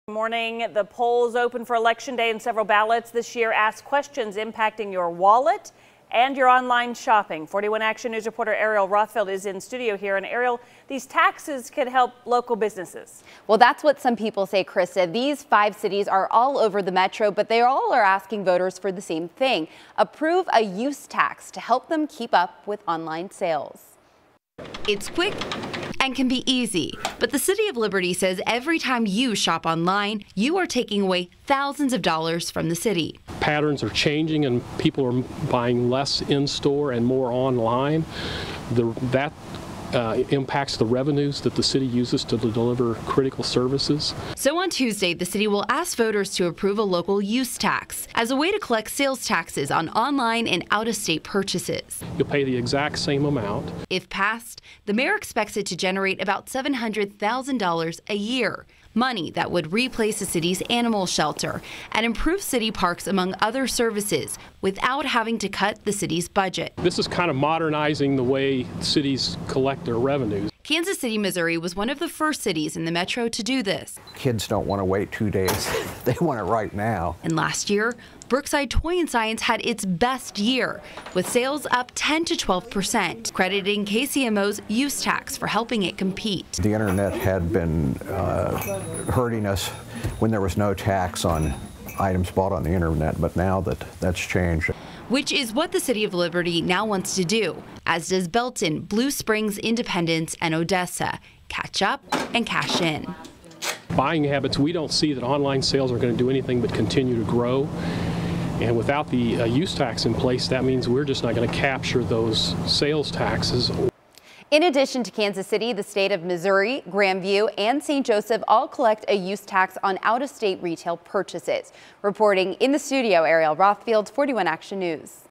Liberty voters to decide on online sales tax
Whenever you purchase an item online and send it to Liberty, the city loses out on sales tax money from that purchase.